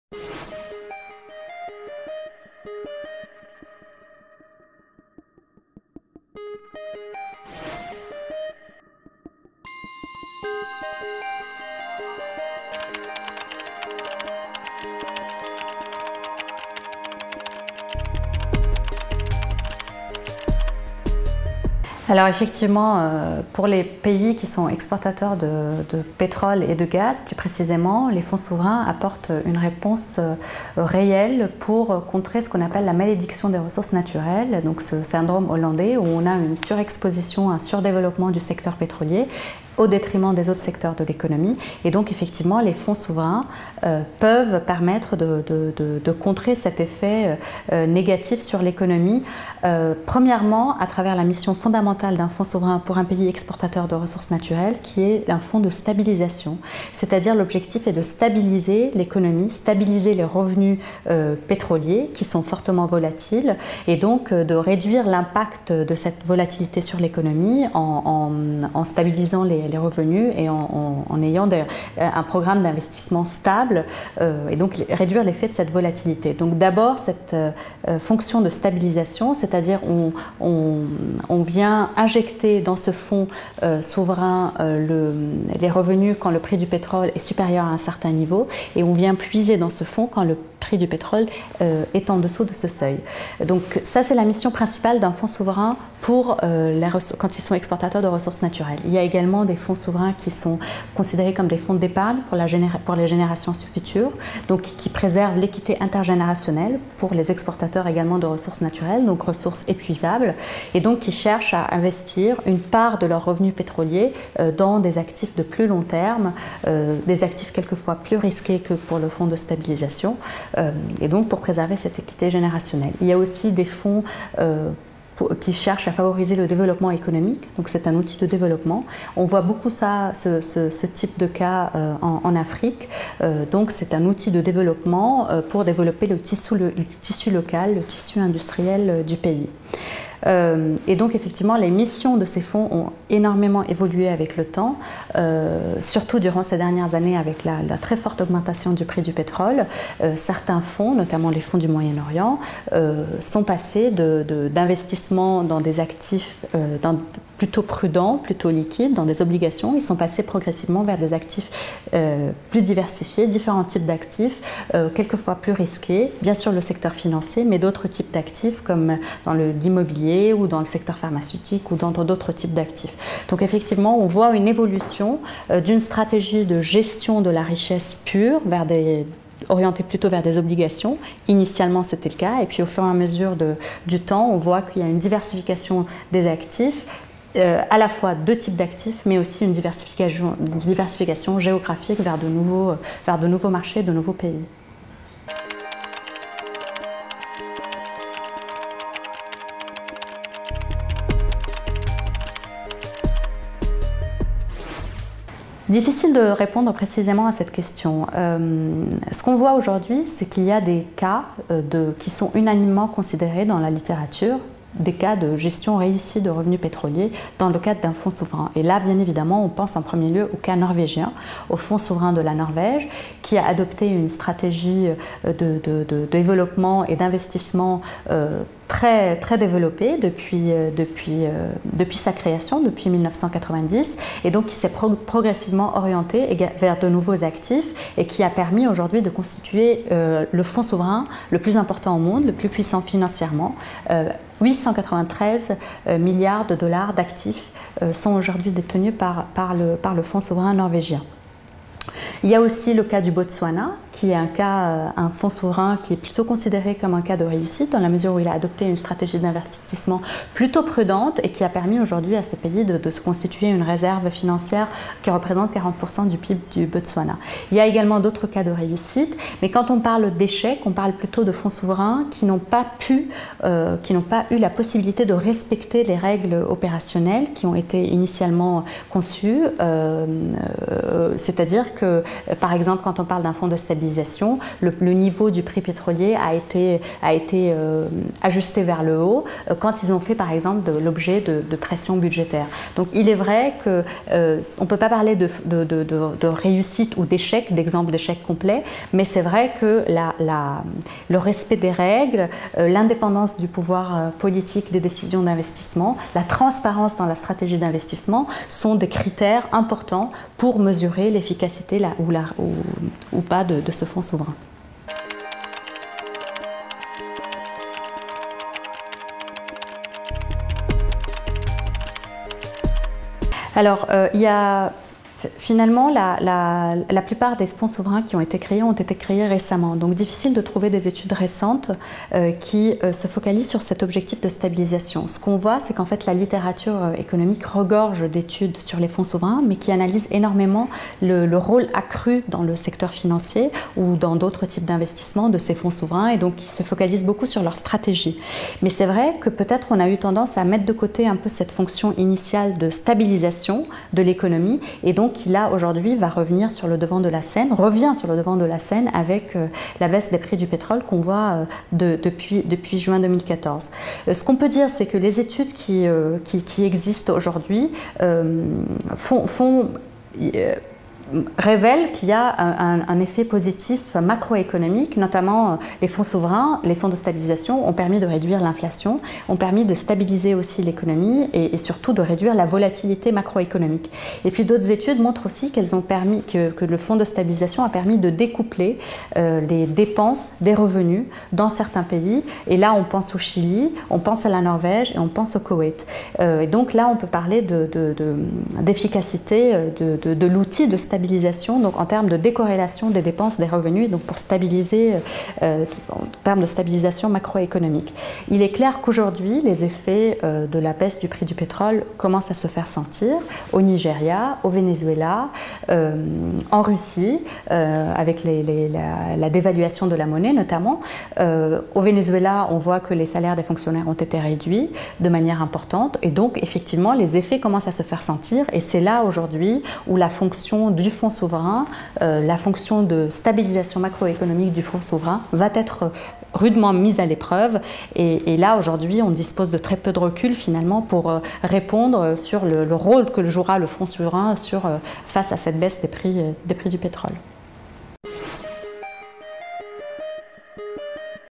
Marie-Claire AOUN, Directrice, centre Energie, Ifri
Les ressources énergétiques et les enjeux de la gestion de leurs revenus